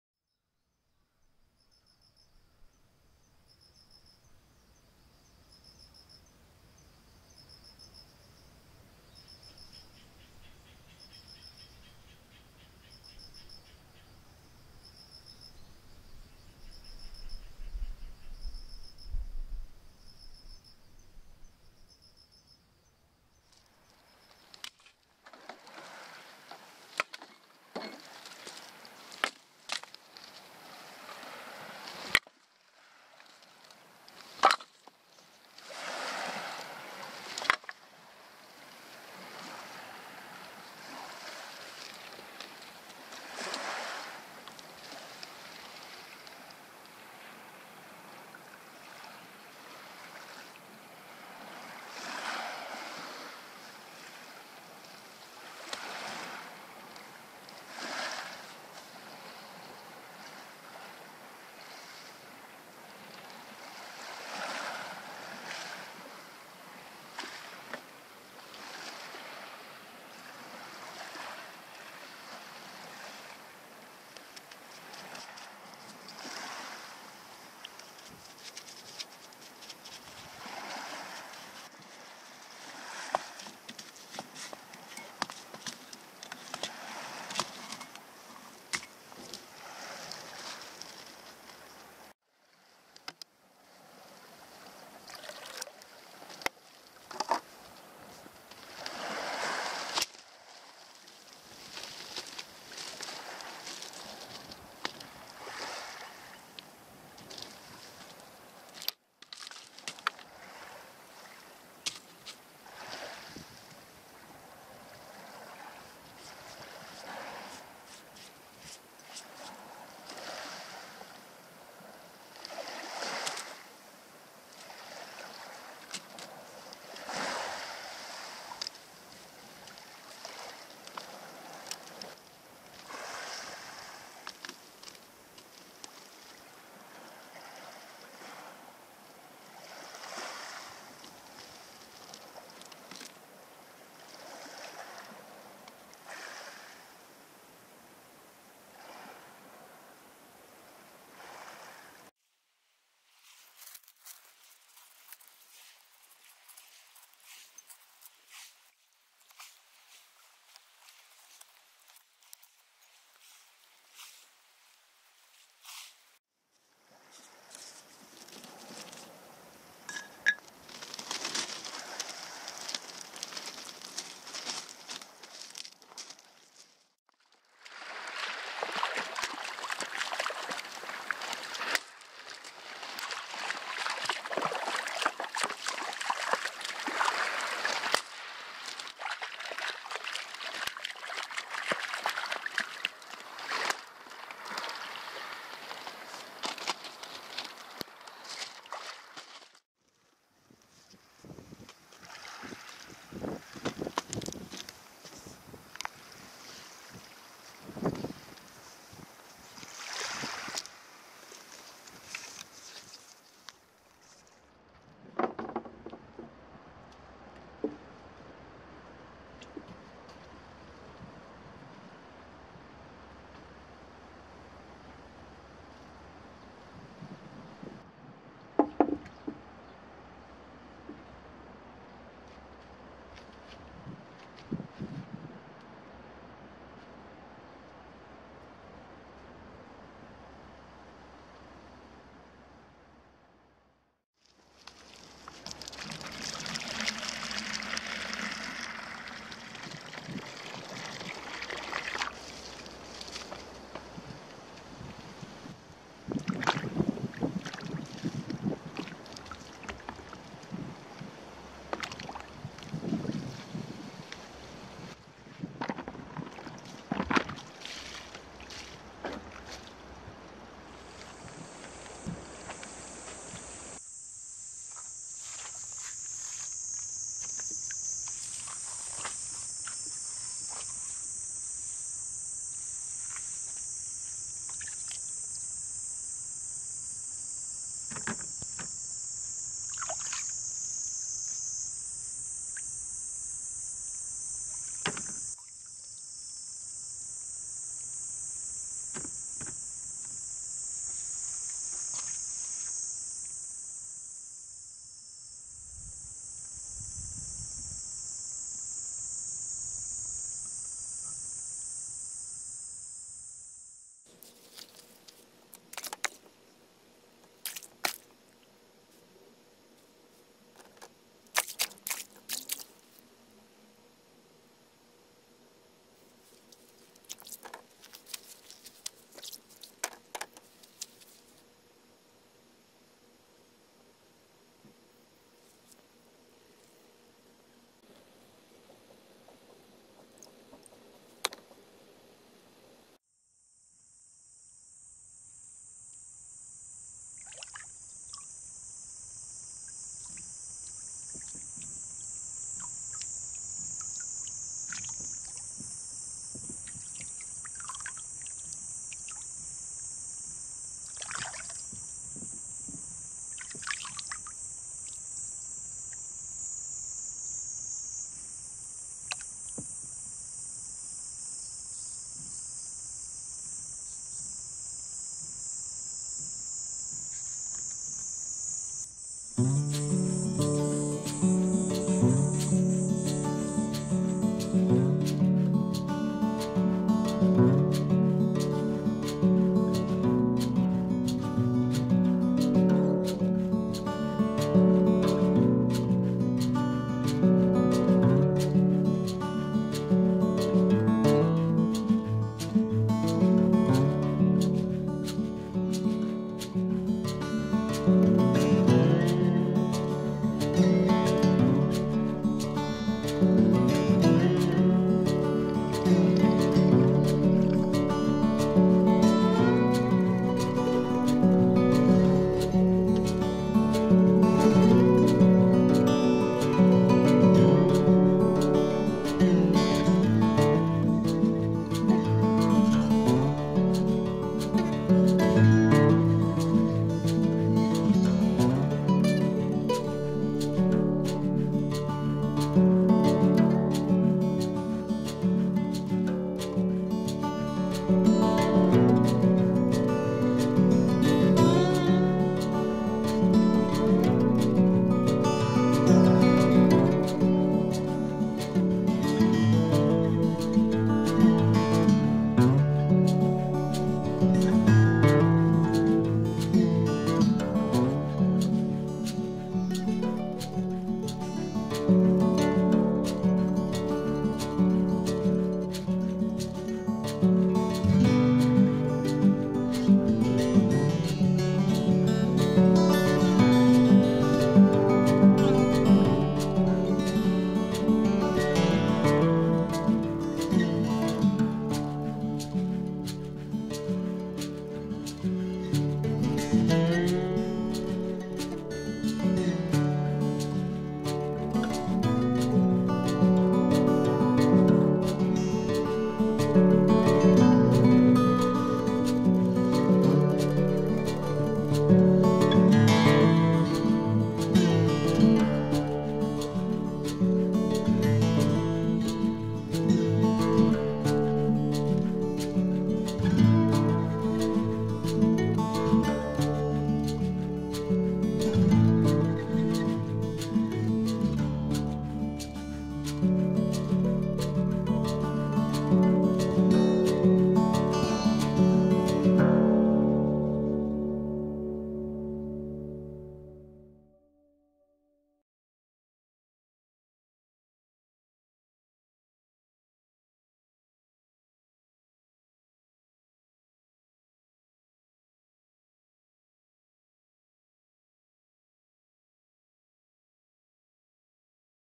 【柿渋 de ASOBO】アクアリウム用の『黒褐色の流木』を作ってみた
柿渋（かきしぶ）を使って流木を黒褐色に仕上げる方法を紹介します。今回はメダカ水槽に入れてみました。